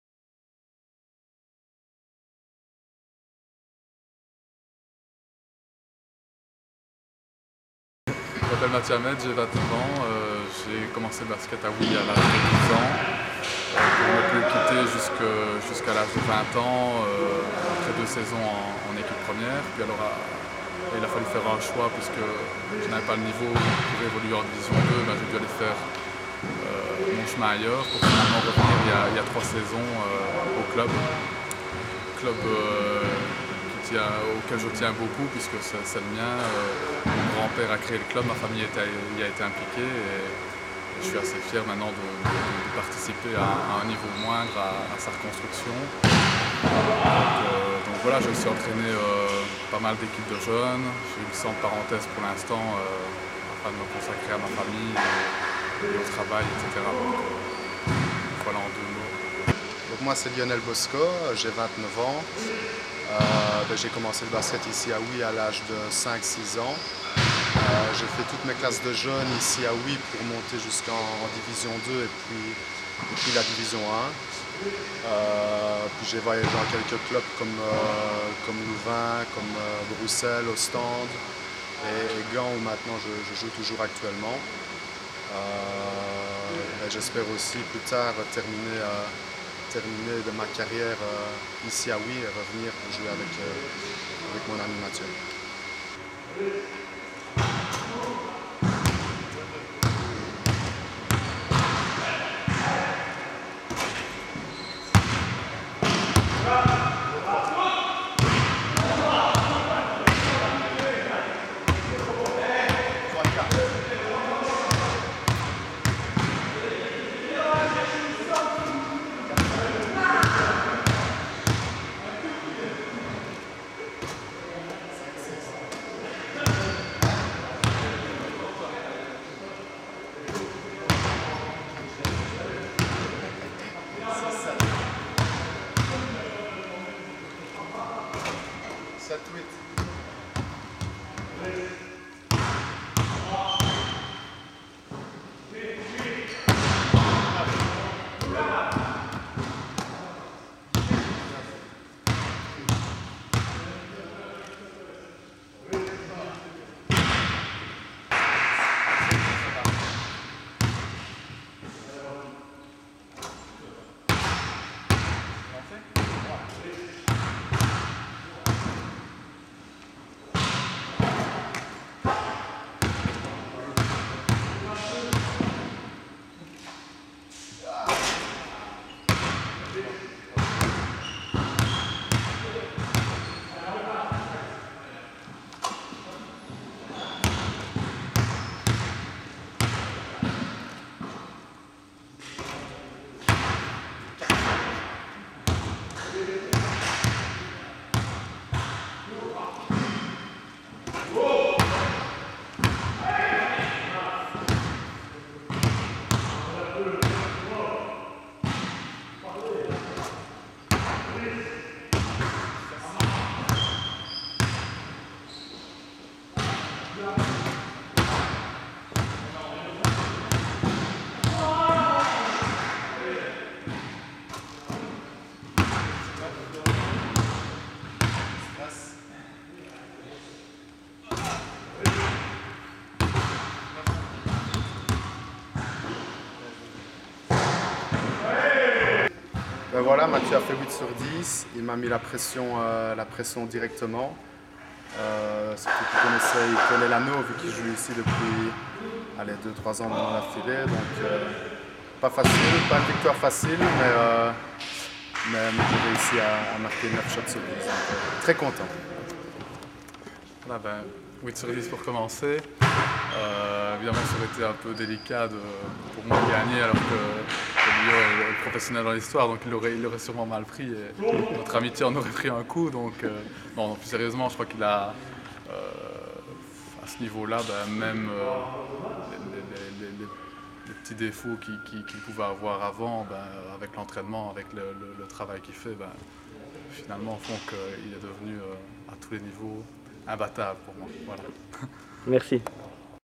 70 ans de Huy Basket
A l'occasion des 70 ans de Huy Basket, la Meuse Huy-Waremme a réuni Lionel Bosco et Mathieu Hamaite. Les deux amis se sont livrés à un concours de lancers francs à déguster avec délectation.